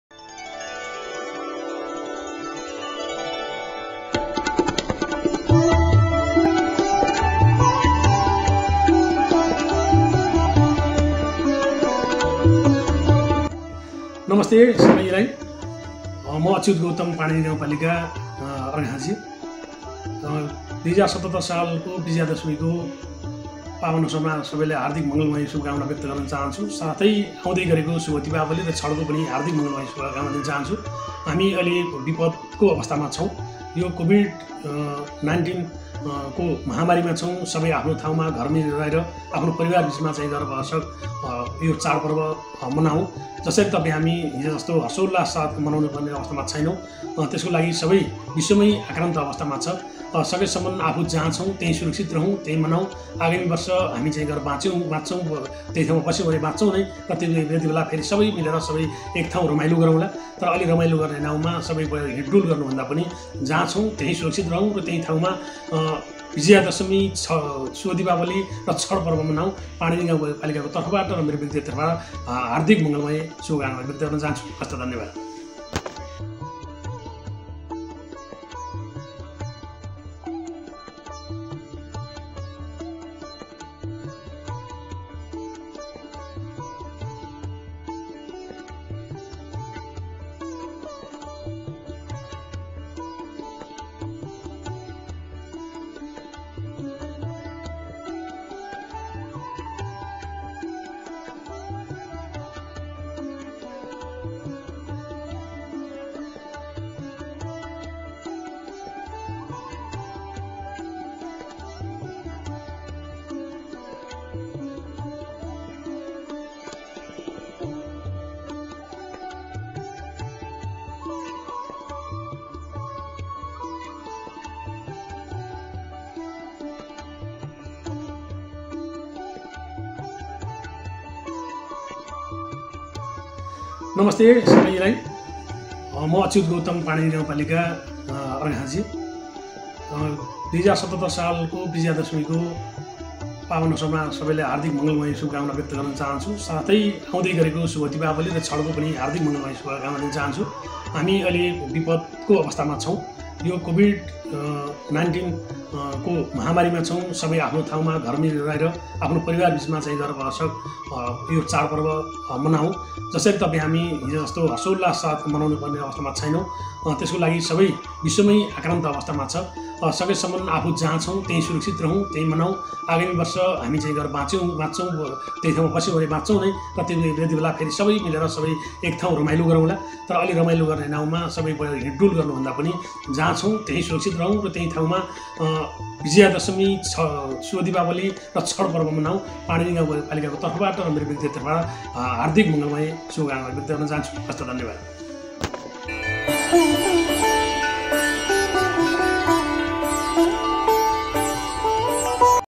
पाणिनी गाउँपालिकाका अध्यक्ष श्री अच्युत गौतम ज्युद्धारा दशैं, तिहार र छठको शुभकामना
पाणिनी गाउँपालिकाका अध्यक्ष श्री अच्युत गौतम ज्युद्धारा विजया दशमी, तिहार र छठ पर्व २०७७ को शुभकामना !!!

पाणिनी गाउँपालिकाका अध्यक्ष श्री अच्युत गौतम ज्युले महान चाड शुभ विजया दशमी, तिहार र छठ पर्व २०७७ को अवसरमा सम्पूर्ण गाउँपालिका कर्मचारी, छत्रदेवबासी, जिल्लाबासी र देश विदेशमा रहेका समस्त नेपालीहरुमा हार्दिक शुभकामना व्यक्त गर्नुभएको छ ।  - अर्घाखाँचीडट्कम